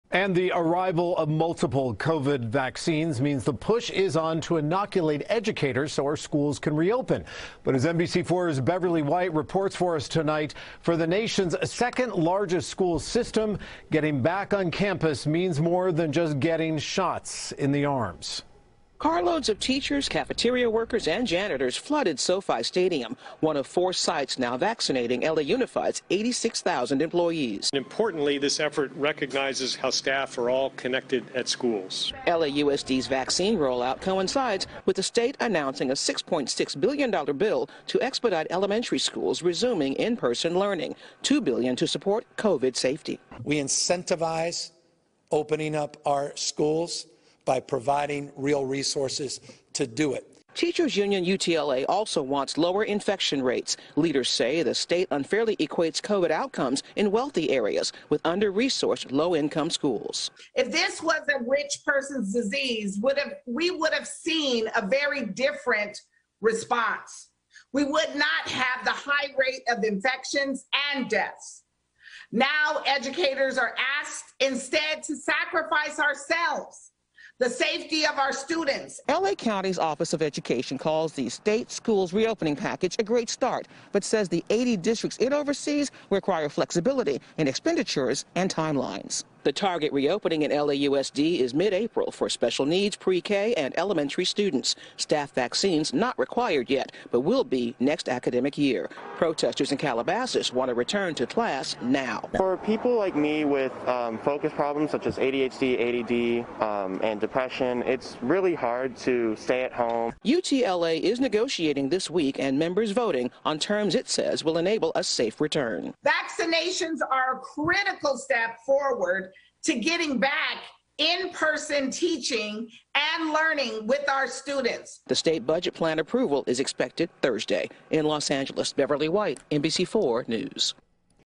LAUSD Getting Back to In-Person Teaching Not As Easy As Shots In Arms | NBCLA
Along with vaccines, the teachers’ union wants to see lower infection rates before educators return to the classroom. Beverly White reported on NBC4 News on Monday, March 1, 2021.

About NBCLA